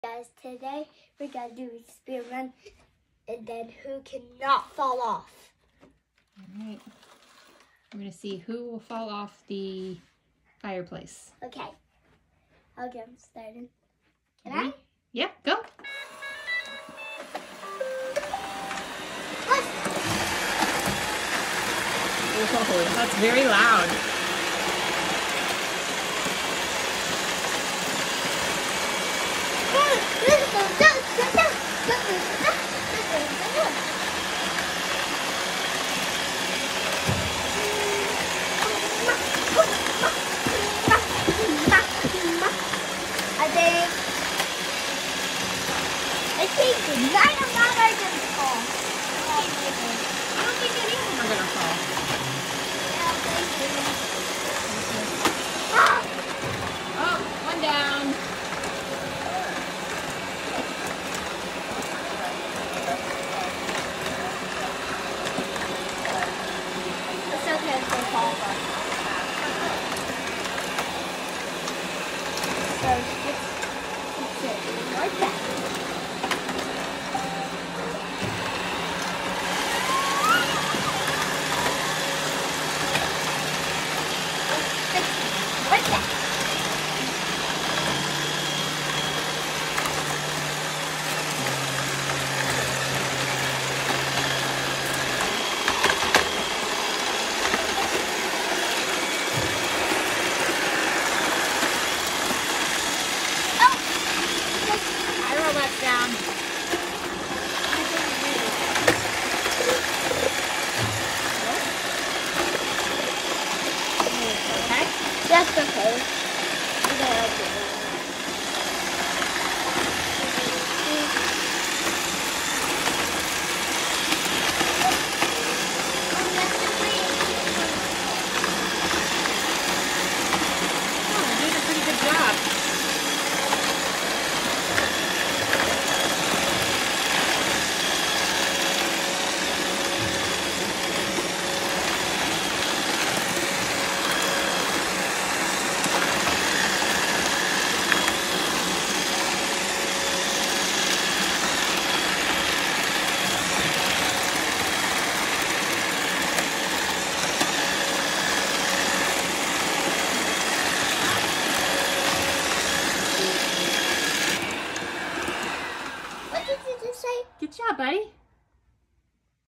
How to clean dirty soot from the front of the fireplace… with 4 Robot Vacuums!!!! Will they fall???
Today, Wyatt experiments with the robot vacuums in front of the fireplace!! Can they clean up the soot? And will they fall over the edge?